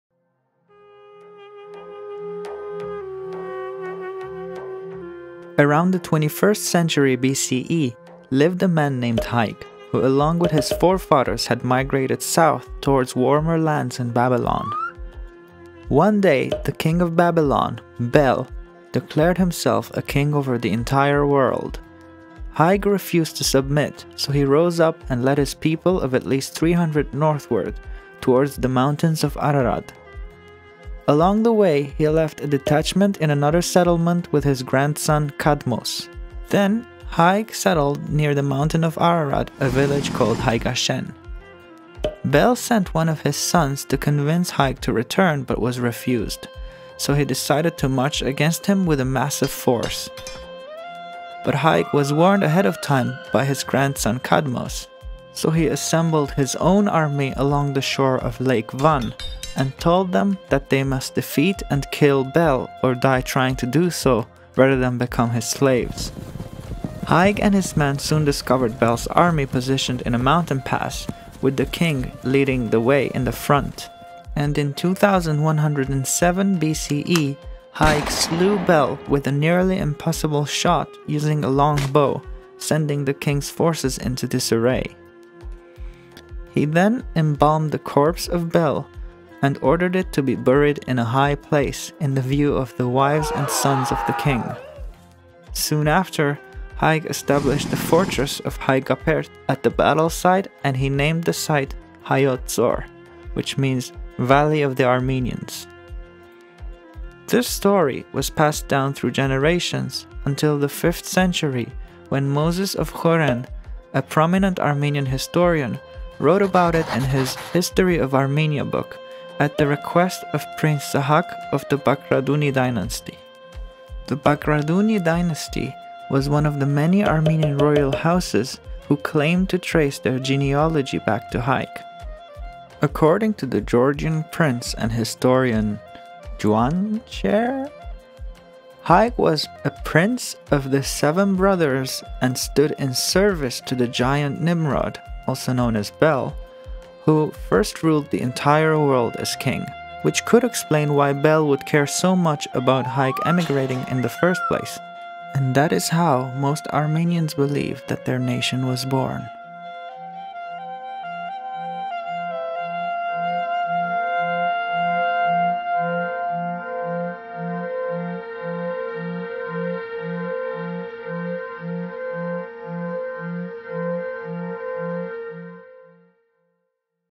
The Epic Legend of Hayk
Had to start somewhere, why not at the beginning?
The first episode(s) may lean towards folklore, Armenia is a very old nation and the sources tend to get blurry this far back. I plan to take the channel towards a more factual and historical direction.

Chapters:
00:00 The Legend
01:46 The History